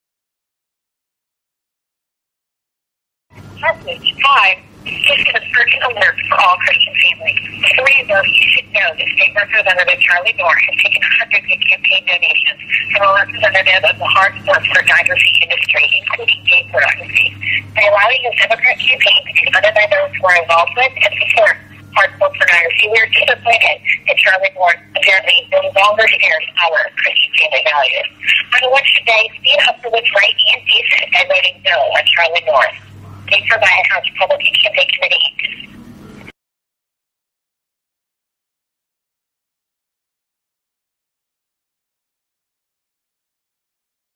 HRCC on State Rep. Norr
State Rep. Charlie Norr was the target of a robocall by the Missouri House Republican Campaign Committee.